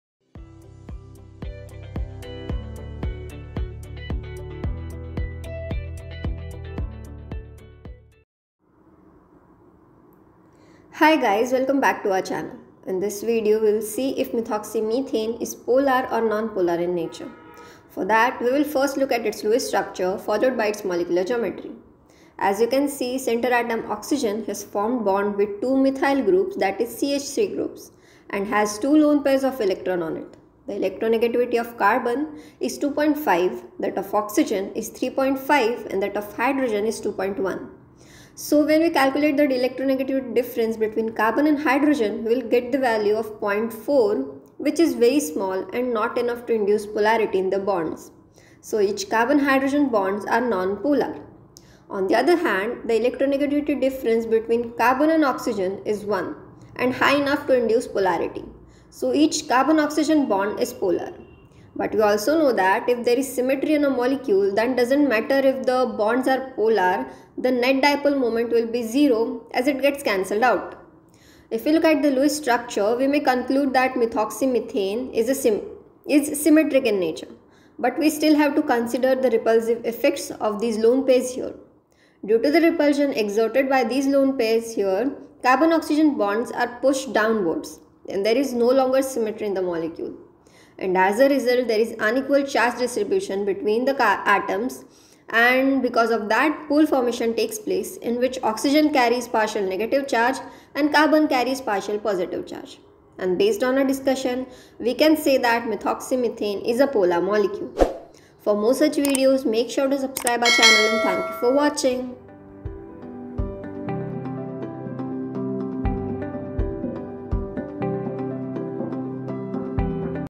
CH3OCH3 Polar Or NonPolar: Check Polarity
Today in this video, we are going to determine the polarity of a CH3OCH3 molecule. CH3OCH3 is a chemical formula of methoxy methane.

To find out if the molecule is polar or nonpolar, we will look at its molecular structure and see its arrangement. We will further look at the electronegativities of the atoms and check if the bonds formed in this molecule are polar or nonpolar.